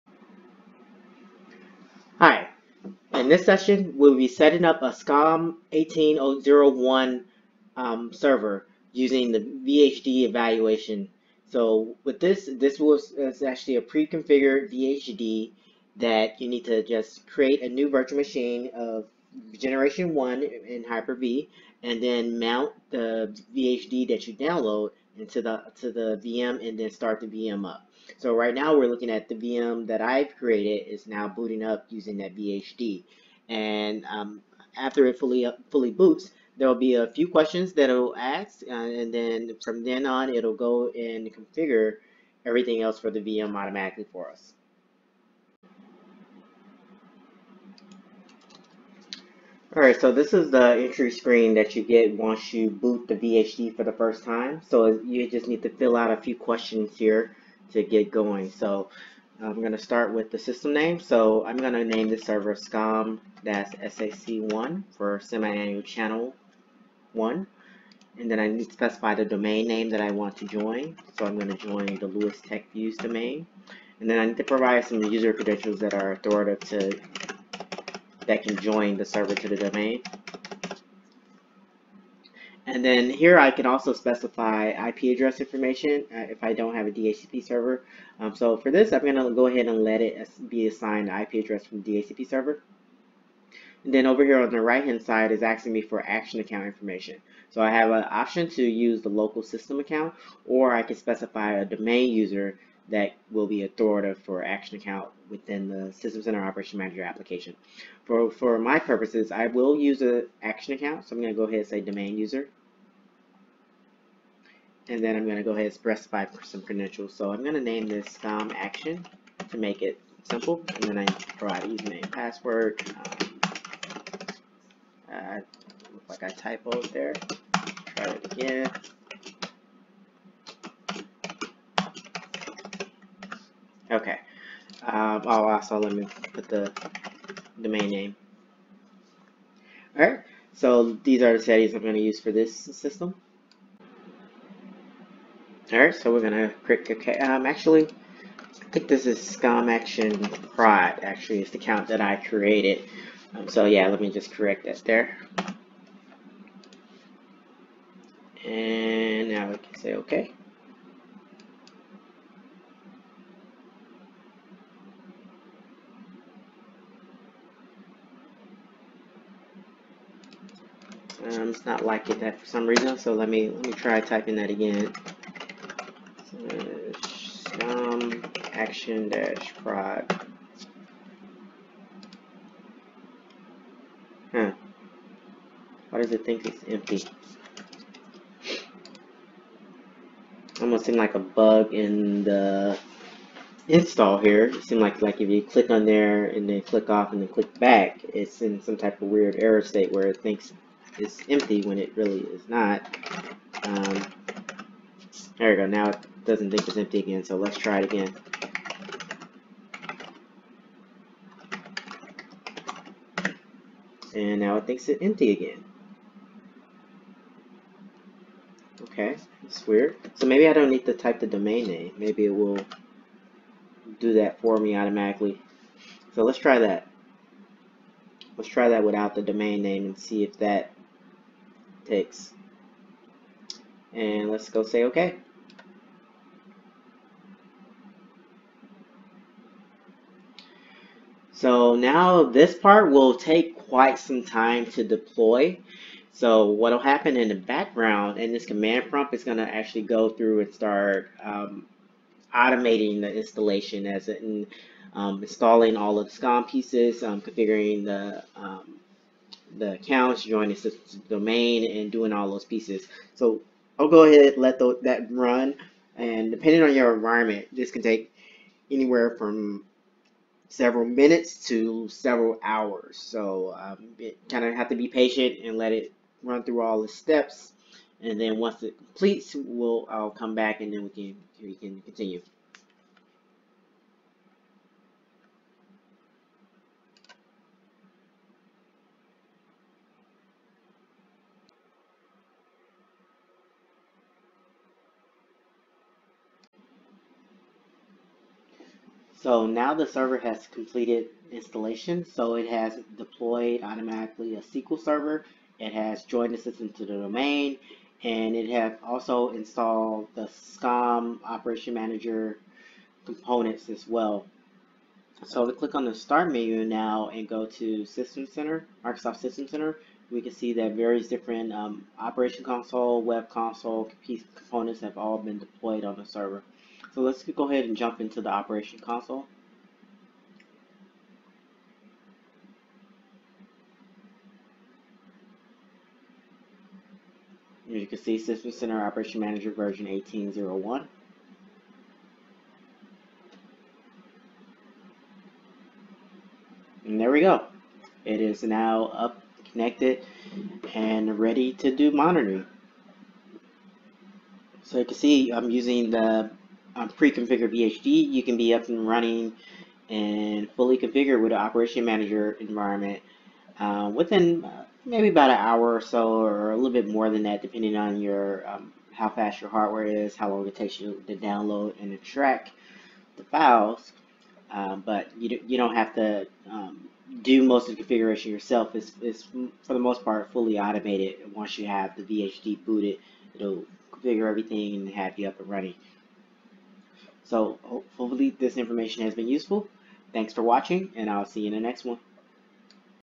Lab Build Out - Part 8 - Installing SCOM 1801 from VHD Eval
In this session, we will be installing System Center Operations Manager (SCOM) 1801 from the VHD Evaluation.